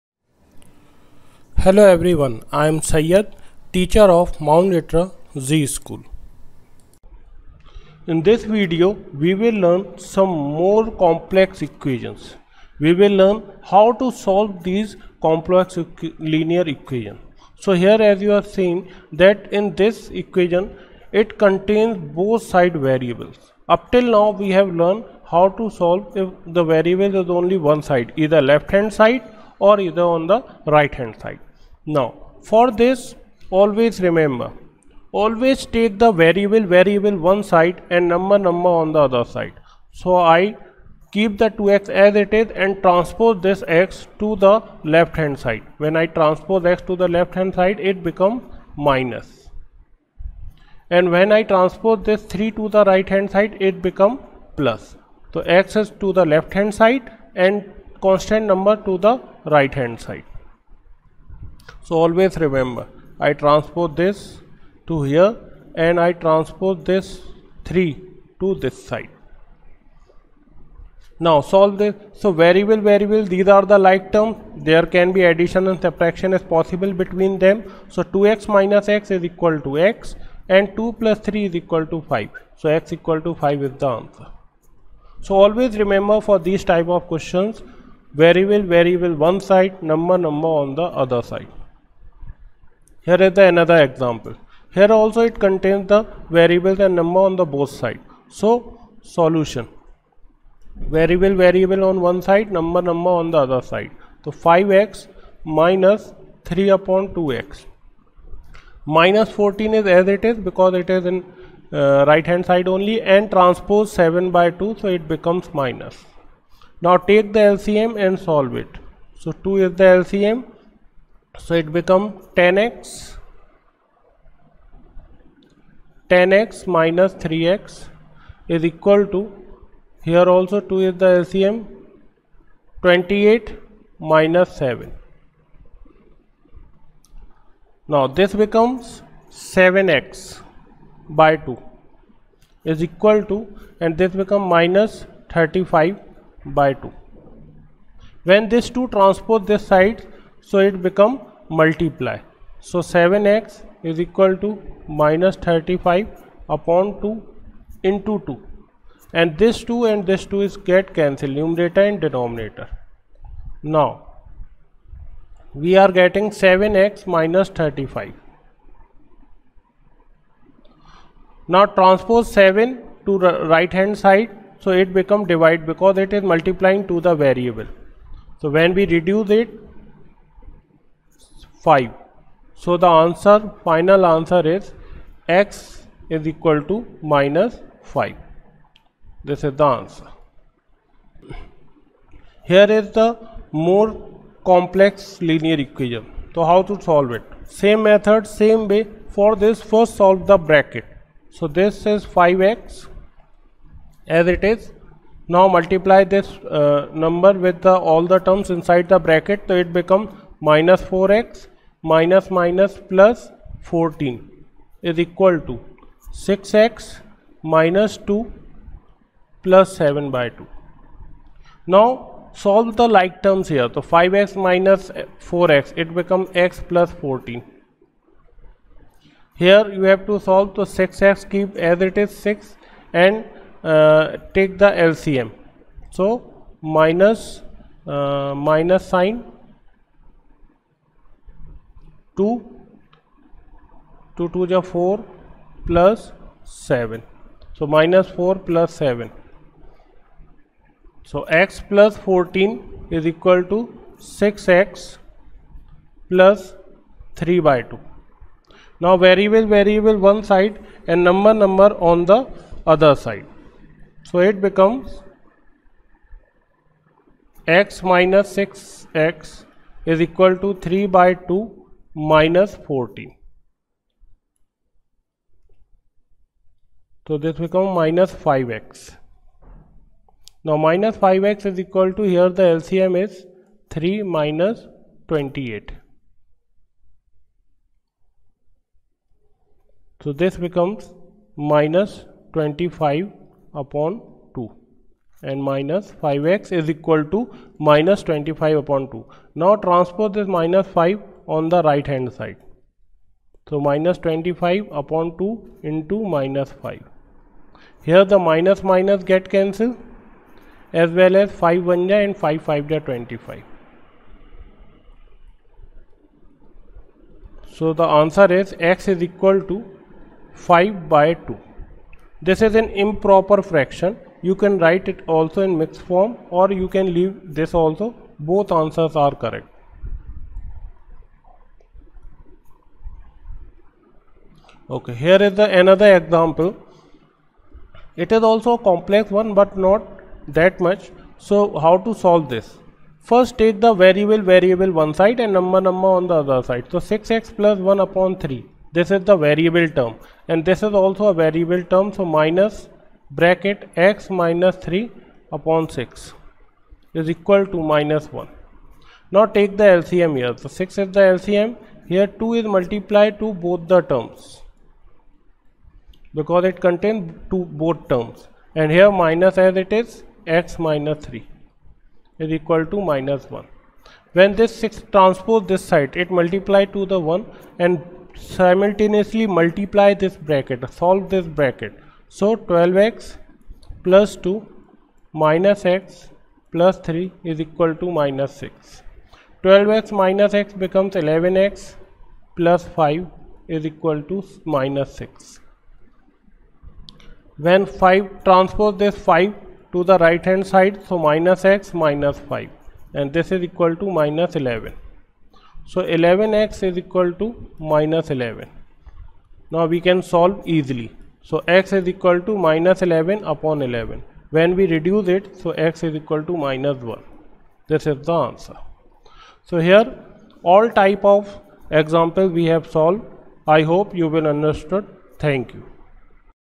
MLZS JHANSI CLASS 8 Mathematics Chapter 2 Linear Equations Lecture 2
MLZS JHANSI CLASS 8 Mathematics Chapter 2 Linear Equations Lecture 2 By Saiyed Sir